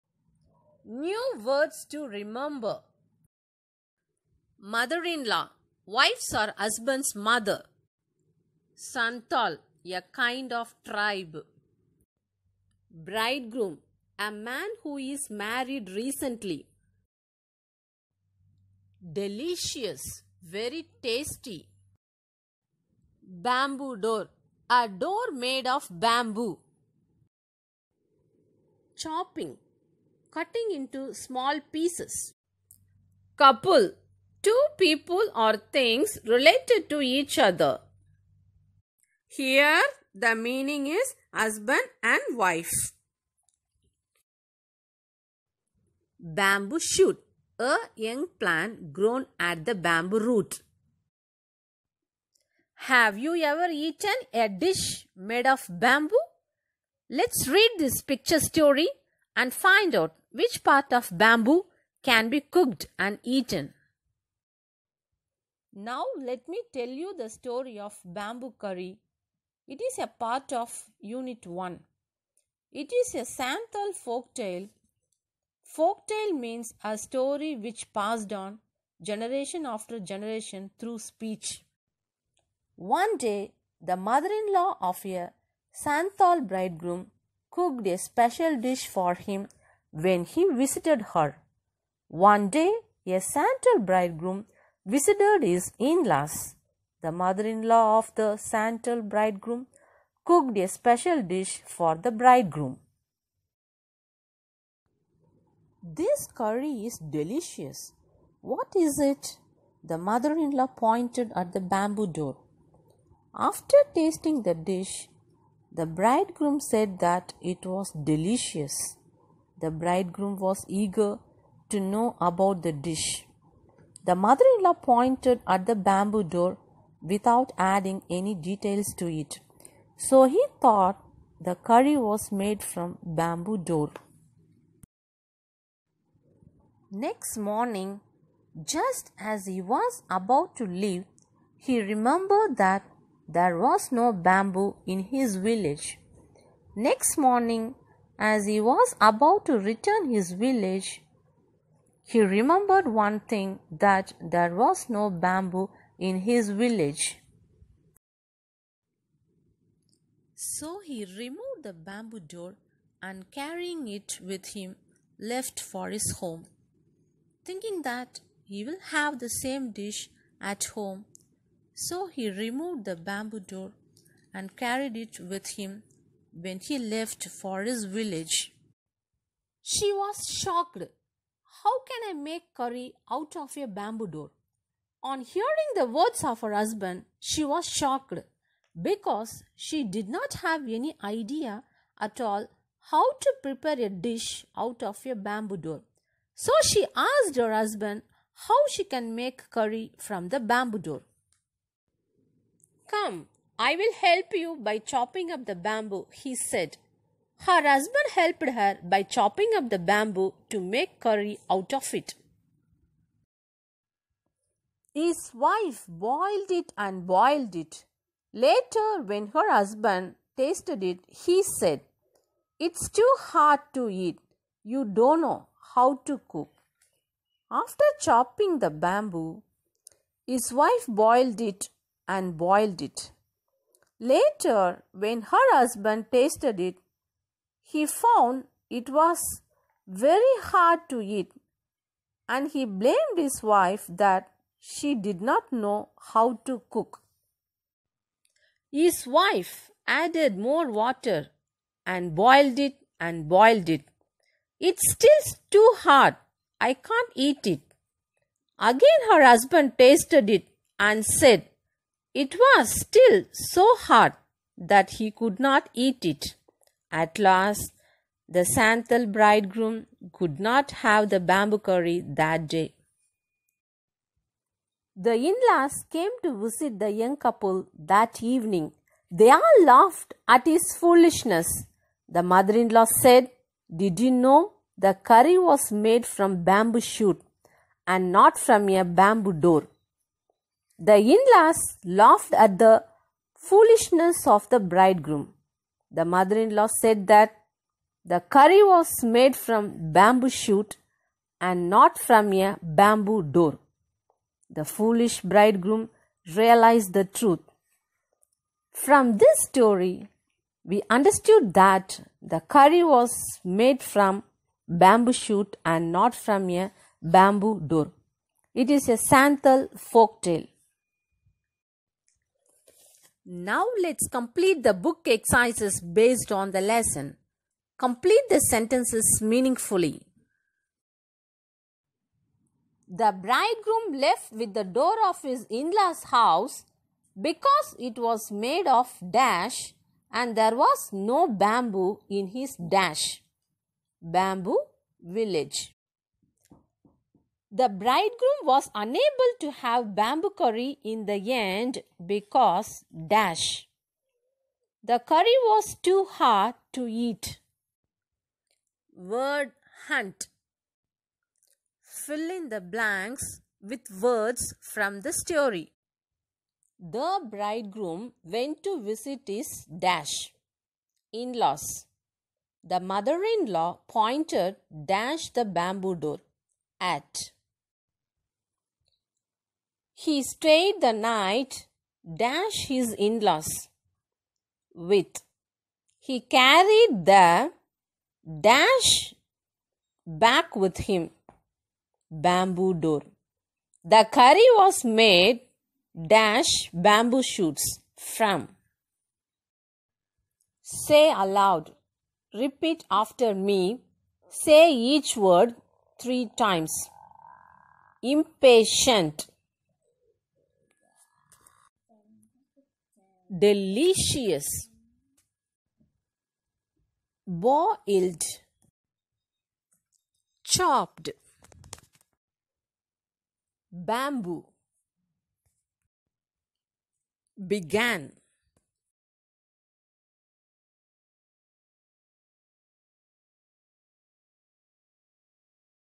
Bamboo Curry Class 5 Unit 1 Explanation in English
Hello viewers! This presentation with picture story  will improve your Listening Skill . It will be beneficial to learn the meanings for odd words from this story.I have also provided the book exercises related to the story which will check your understanding of this lesson.
*Narration: B.Dorothy - TGT, M.A, B.Ed,M.PHIL(ENGLISH)